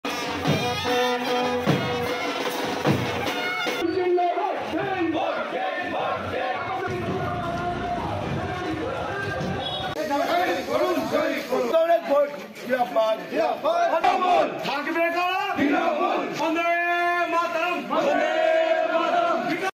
বীরভূম জেলায় পুরভোটের প্রচারে তৃণমূল কংগ্রেস
বীরভূম জেলায় রাজ্য সরকারের বিভিন্ন প্রকল্পের পরিসংখ্যান তুলে ভোট প্রচারে তৃণমূল কংগ্রেস।